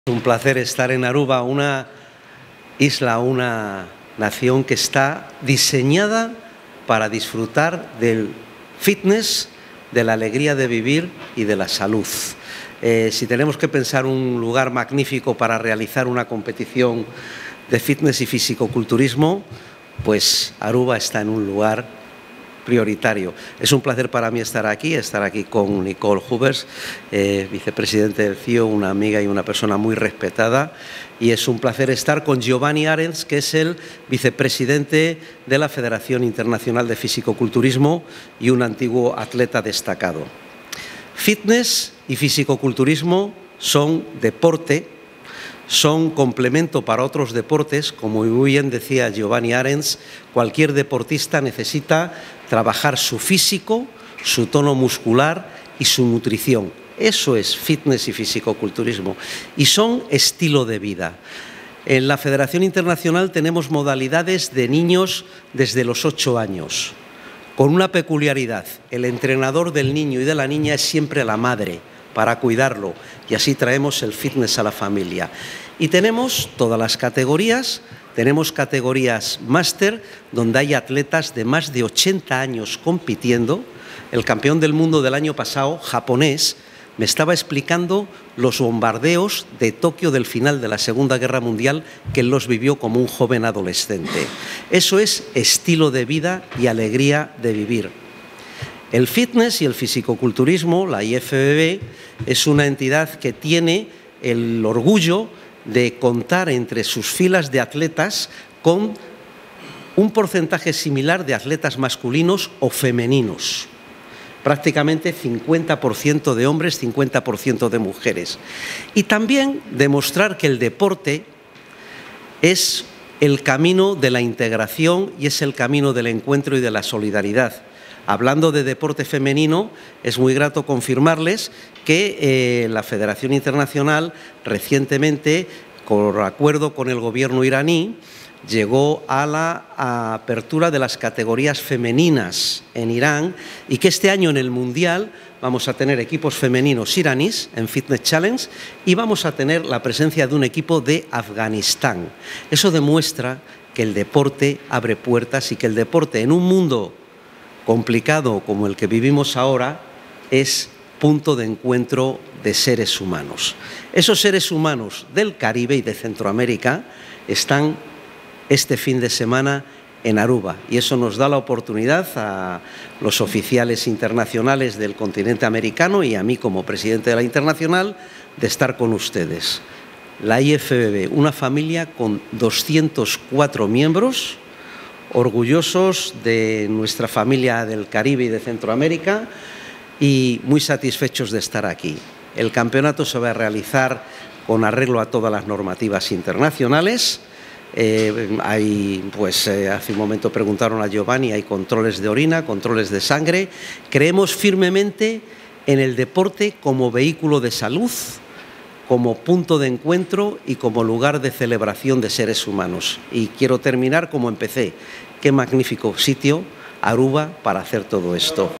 Dr. Rafael Santoja (IFBB): Deporte ta un punto di encuentro cu ta habri hopi porta
Den fin di siman binidero, Aruba lo ta sede di un campeonato grandi di bodybuilding, e Central American & Caribbean Championships.